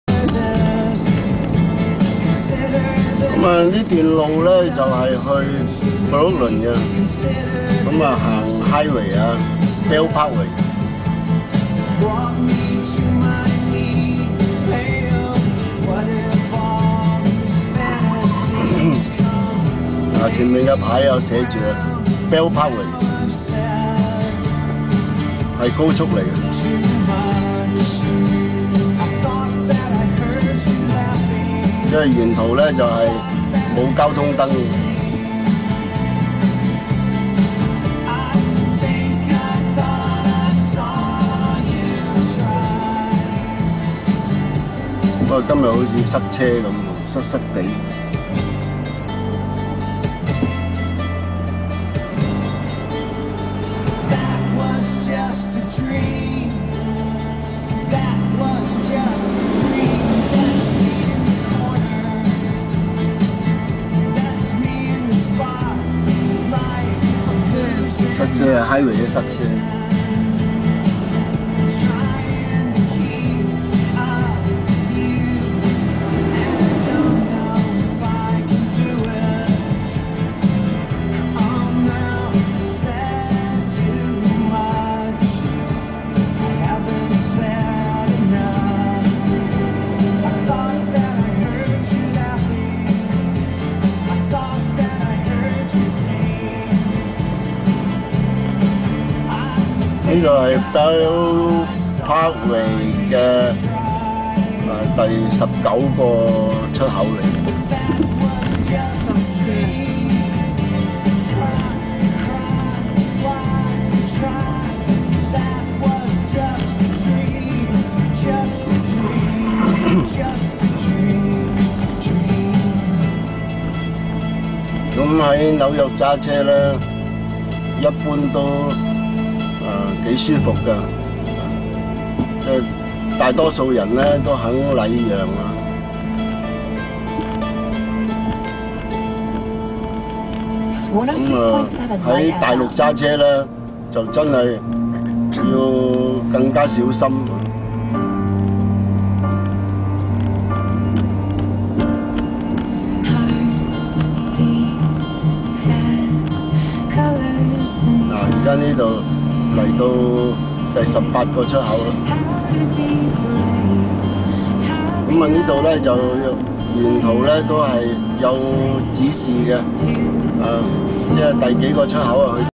Belt P-Way .. Queens to Brooklyn New York USA
Belt P-Way .. Queens to Brooklyn New York USA from Exit 19 to Exit 11N (Ave U King's Plaza) ..